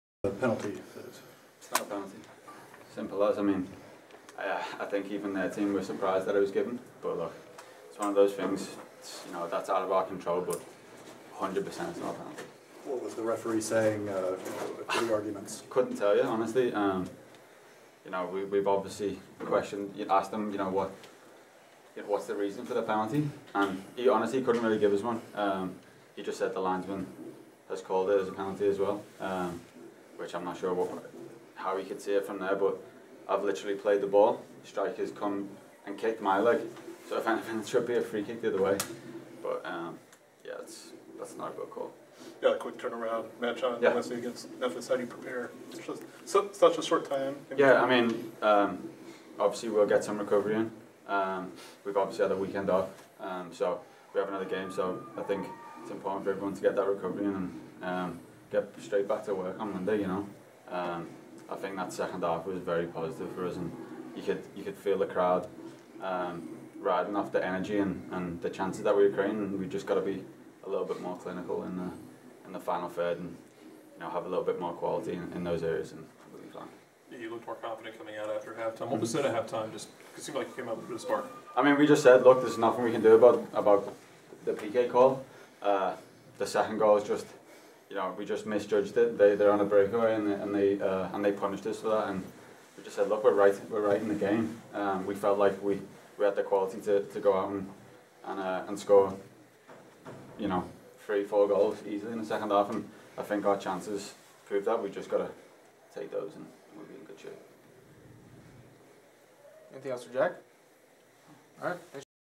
Original Free Kick: Atlanta United 2 Jack Metcalf post-Saint Louis presser (4.6.19)
The Sports Inquirer attended the presser of Atlanta United 2 captain Jack Metcalf following his team's 2-0 home loss to Saint Louis FC on April 6. Topics included the foul called on him in the first half that created a penalty shot opportunity for Saint Louis, Atlanta's better form in the second half and preparing to visit Memphis on April 10.